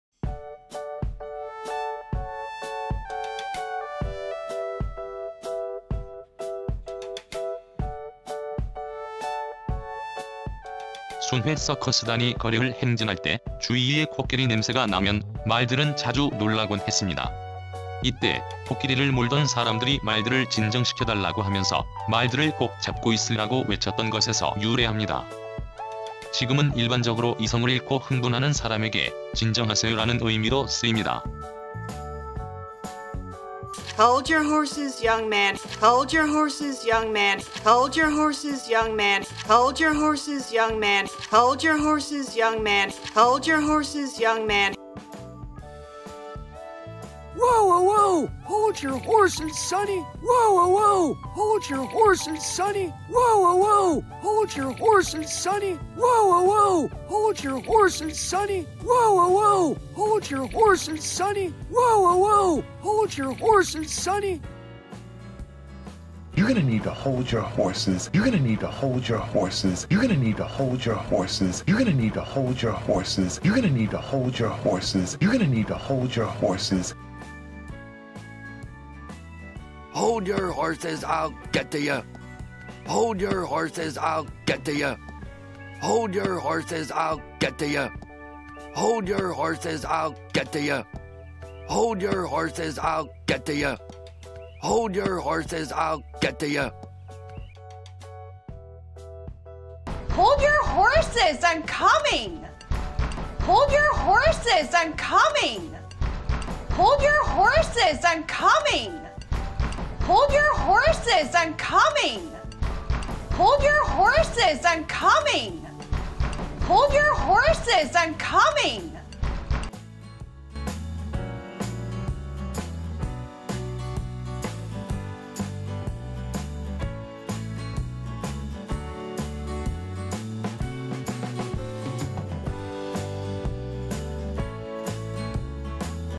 #04. 미드 이디엄 쉐도잉 (hold one's horses)
미드 이디엄을 쉽게 이해하면서 재미있게 학습할 수 있도록 동영상을 제작해 보았습니다.

블로그 : njoycafe.blog.me